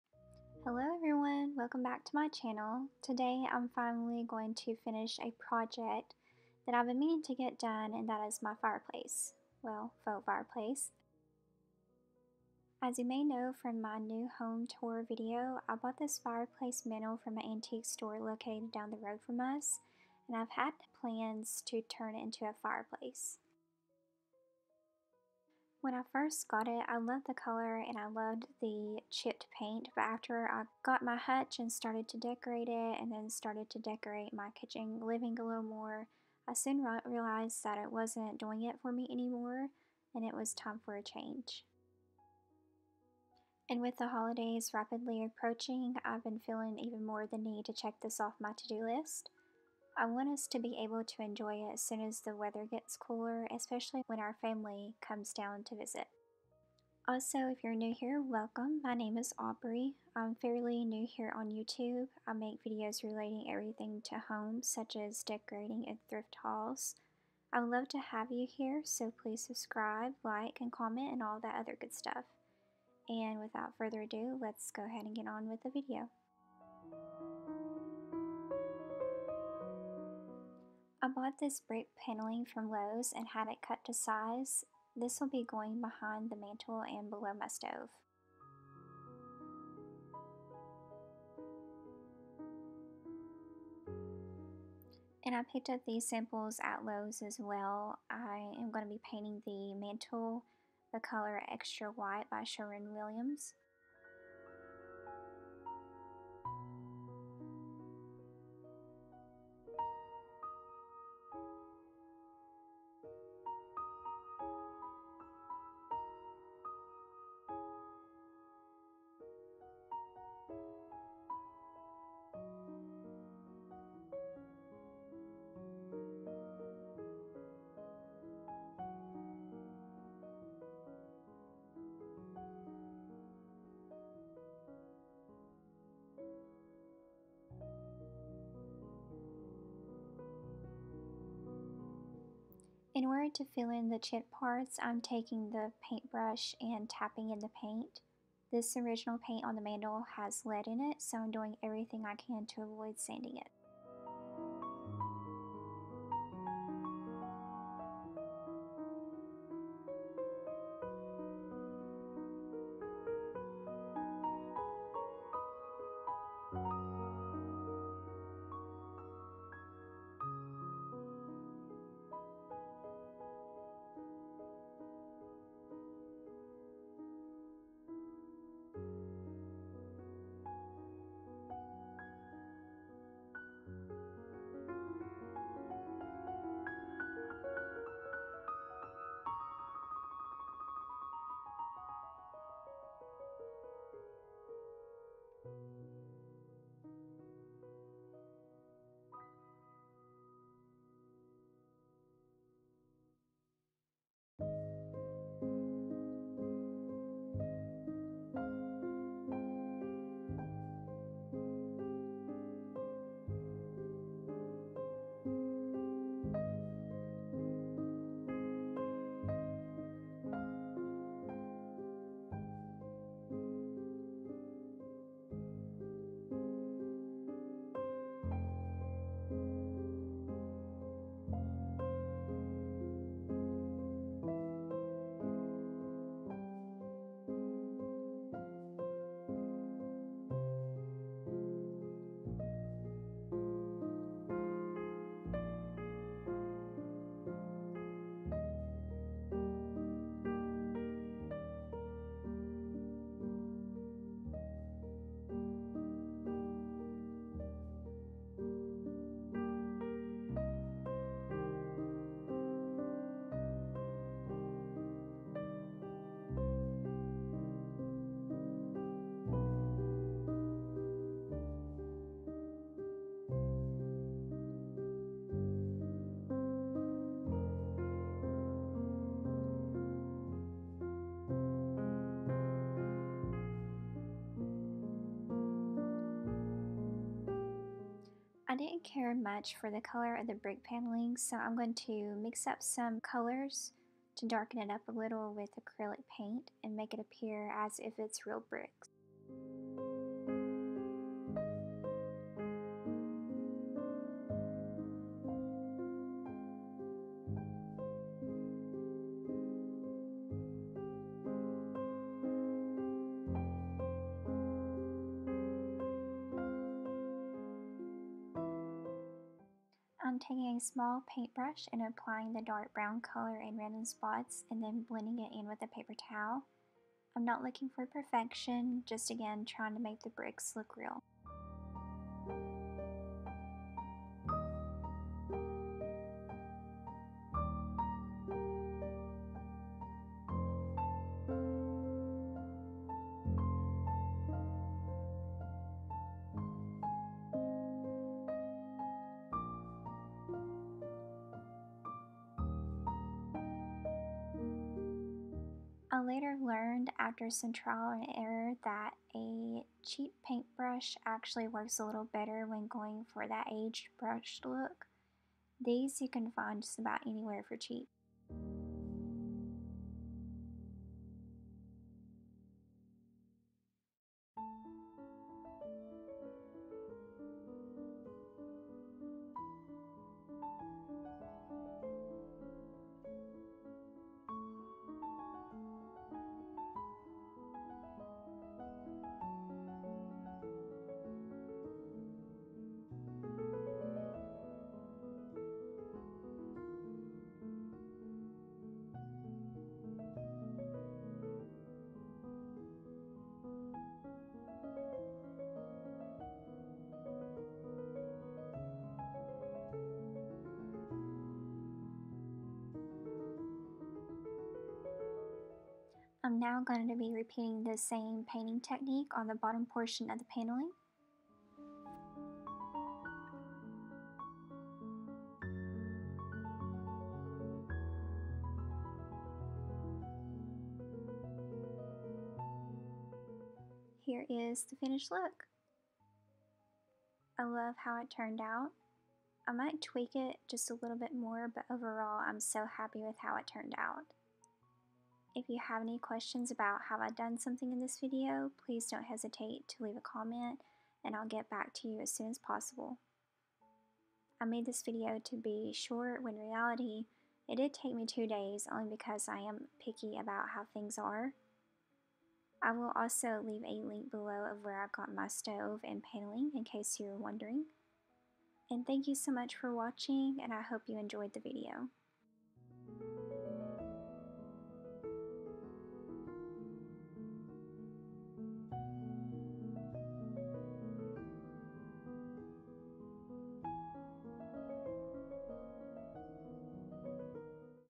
Fireplace Makeover - Budget friendly DIY
Hello Youtube friends! In today's video I am going to be giving my faux fireplace a makeover. When I first bought this mantle, I liked the color and the fact that it was distressed. It's a very old piece. But, I am getting close to having my kitchen living done and the mantle doesn't go with my decor anymore. I will be giving it a fresh coat of paint and will be adding paneling to the back and bottom. I hope you enjoy the video!

Fireplace stove

Brick paneling